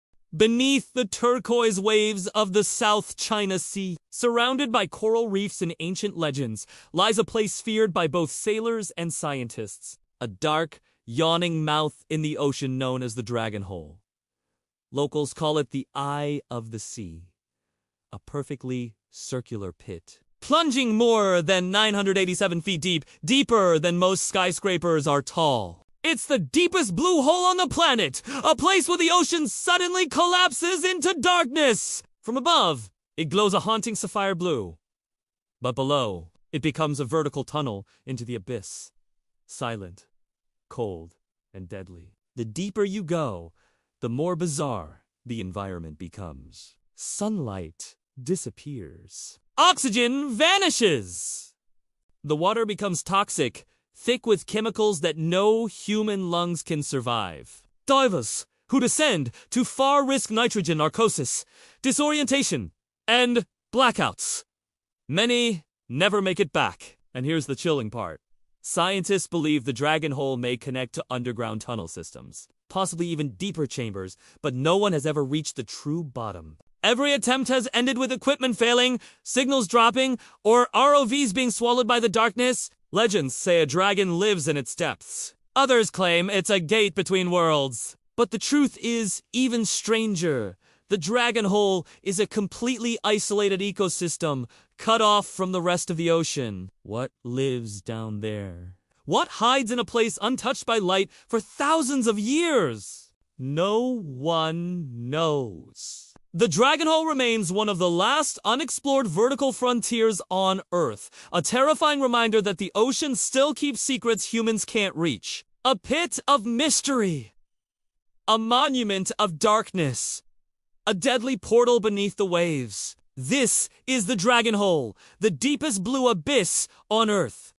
“The Dragon Hole: The Bottomless Pit of the South China Sea 🌊🐉”
In the middle of the South China Sea lies a dark, perfect circle of water known as The Dragon Hole.
It’s the deepest blue hole ever discovered, plunging into an abyss so deep that sunlight dies before reaching the bottom.
For centuries, sailors feared it.
Today, scientists still don’t fully understand it.
Beautiful, mythical, and deadly — this is the Dragon Hole.